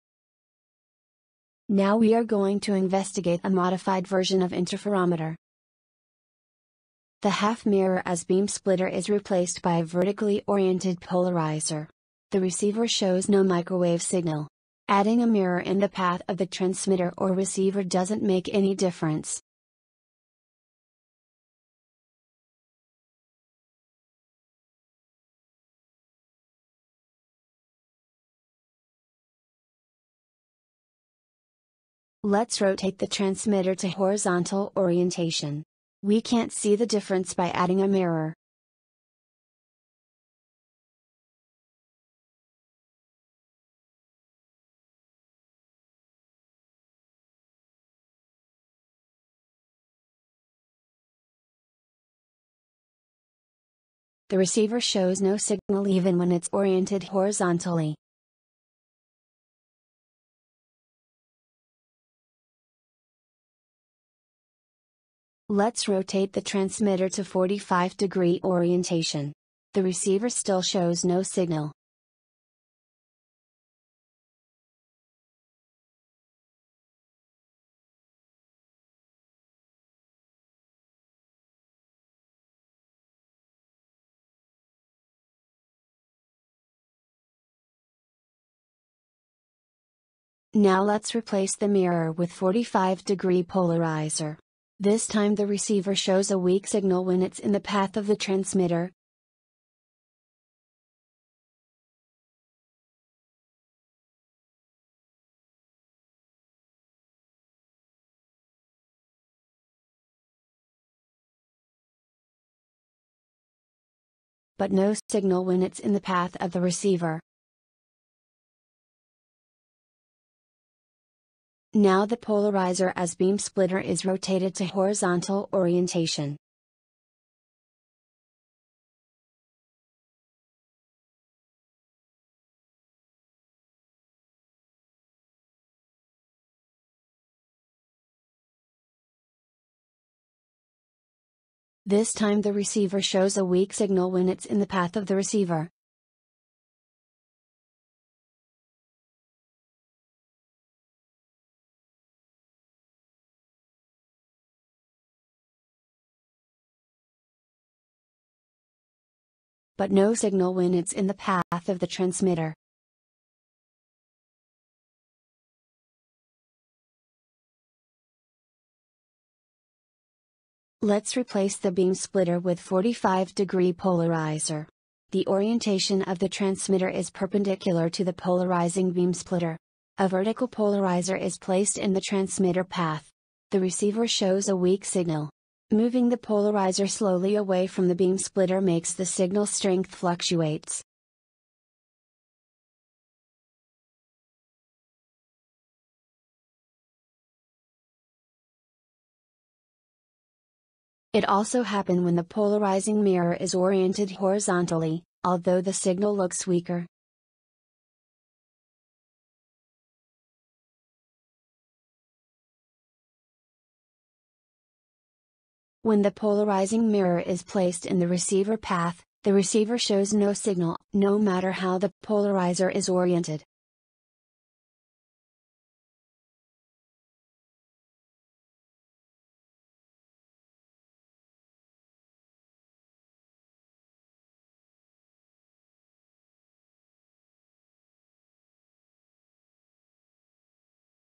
Investigation on Microwave Transceiver 59 : Polarizing Beam Splitter
This video shows a variation of interferometer experiment, in which the beam splitter is replaced by a polarizer. The microwave reflected by the beam splitter is perpendicular to the microwave passed by it.